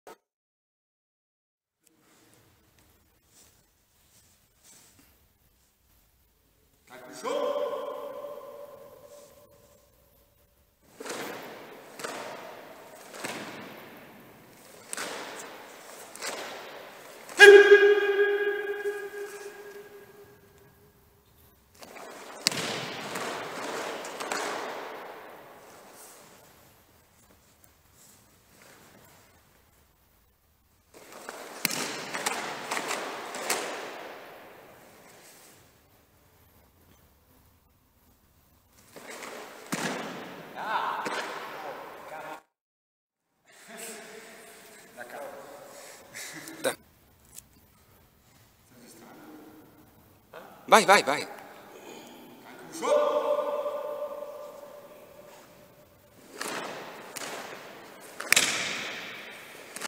Kanku sho #1
Presso la Polisportiva Levata
corso di karate do shotokan, kata Kanku Sho
Victor Adolfo Boni , 4° dan
Nippon Karate Do Academy , diretta da Shihan Shojun Sugiyama 9° dan

piccola riflessione personale : Non sempre riusciamo a far tutto perfetto come vorremmo, tra il pensare alle aspettative e il mantenere la concentrazione ... qualche errore può capitare. Sia come allievi che come maestri non trattiamoci come se fossimo dei robot o delle macchine da combattimento, siam pur sempre persone. :) Oss!